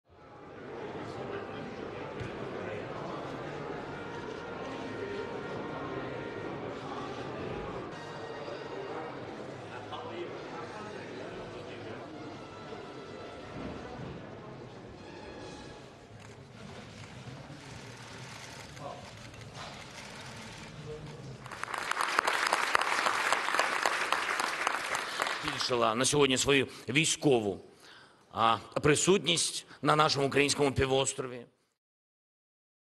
Ukraine richtet Gipfeltreffen zur Zukunft der Krim aus
Die Ukraine richtet in Kiew ein Gipfeltreffen zur Zukunft der annektierten Halbinsel Krim aus. An der Konferenz nehmen dutzende Staats- und Regierungschefs sowie Außenminister teil.